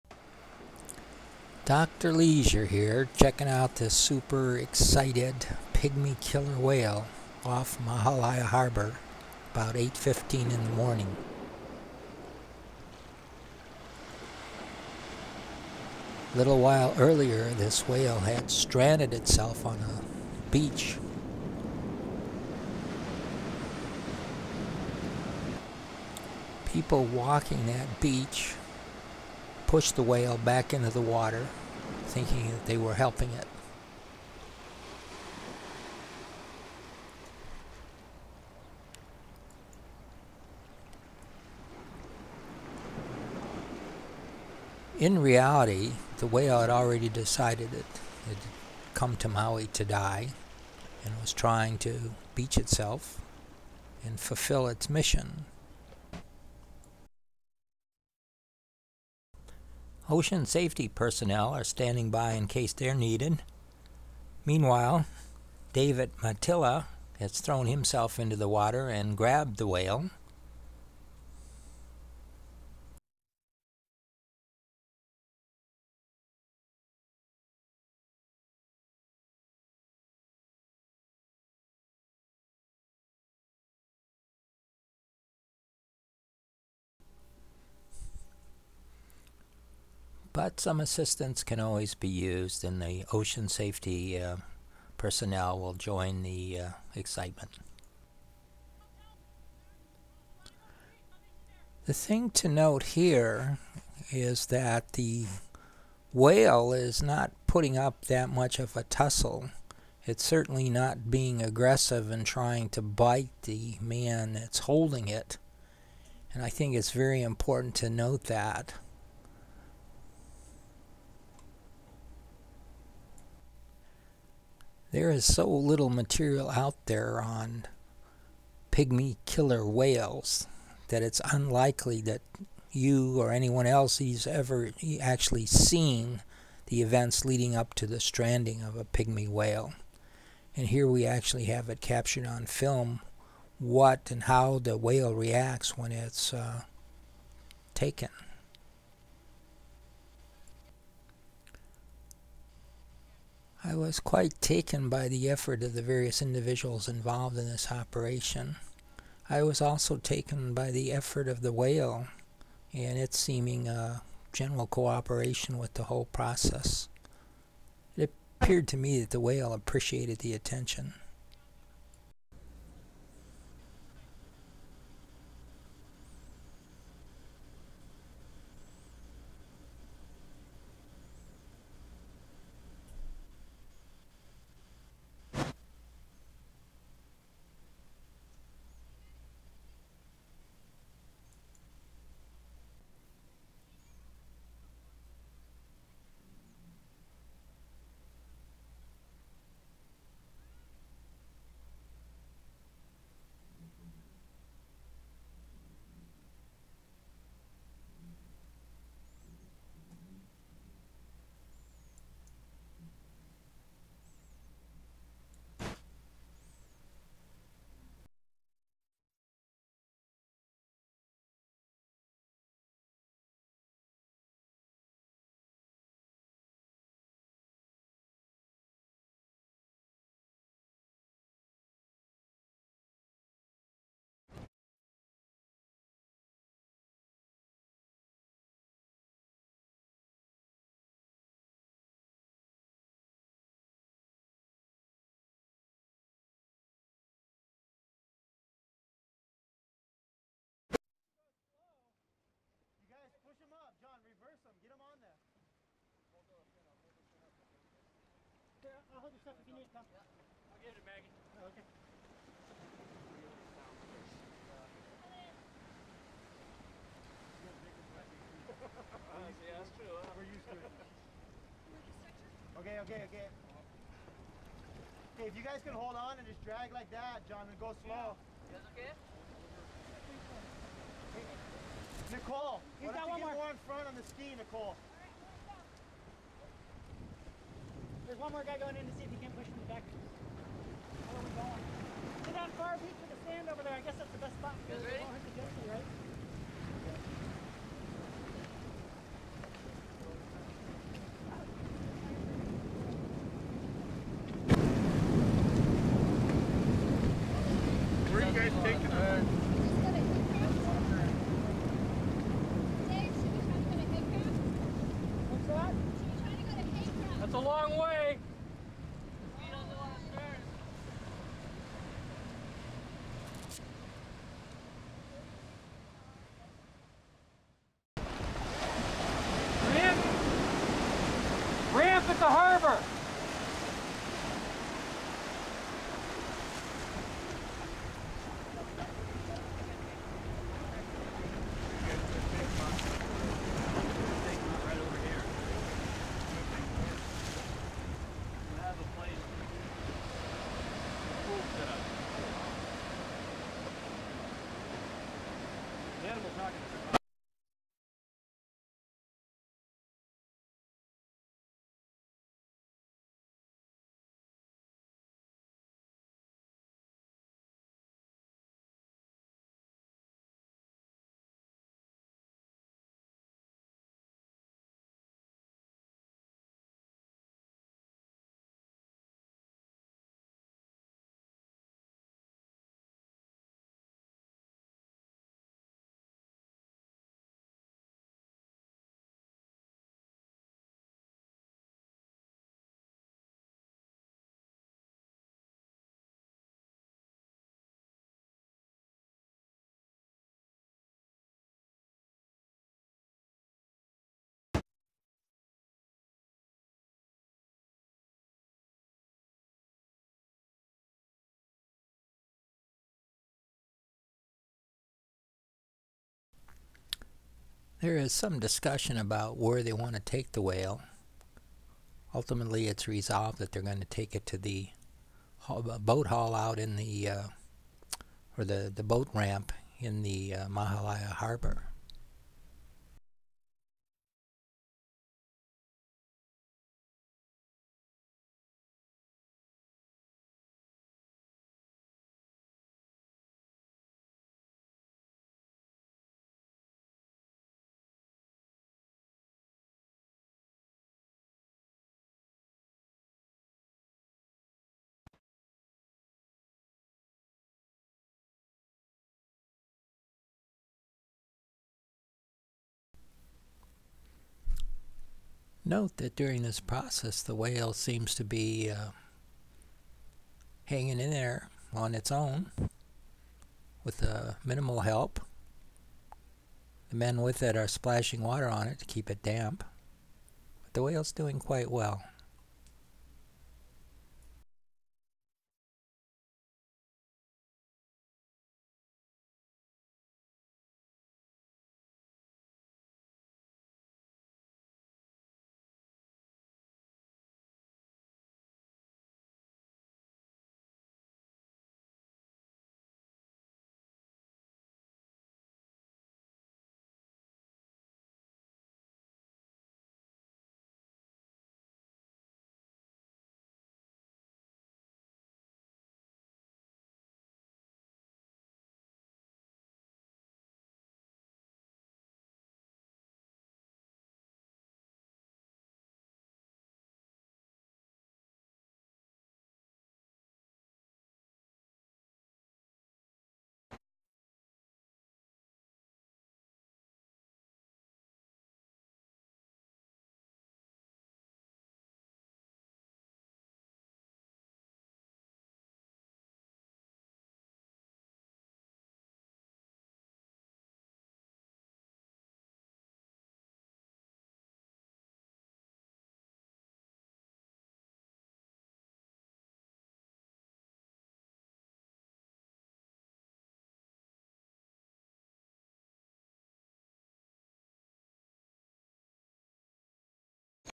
Extraction Pygmy Killer Whale Part I of II
On May 22, 2009 a pygmy killer whale (Feresa attenuata) that had been in the area since May 1, 2009 had beached itself earlier near Maalaea Harbor. People on the beach pushed the creature back into the water not realizing they were interfering with nature's plan. The animal was disoriented and highly agitated and was swimming erratically in the surf off the breakwall at Maalaea Harbor.
NOAA personnel made a decision to capture the whale and assess its situation. Everything suggested that the creature had some serious problems. This was confirmed when blood work and other studies were performed.
There is very little information in the literature regarding the circumstances surrounding strandings of Pygmy Killer Whales. If sighted they are usually seen in water over 500 meters deep. A great deal of information was learned about this species from this stranding. More details to follow. Check out DrLeisure.com for additional information.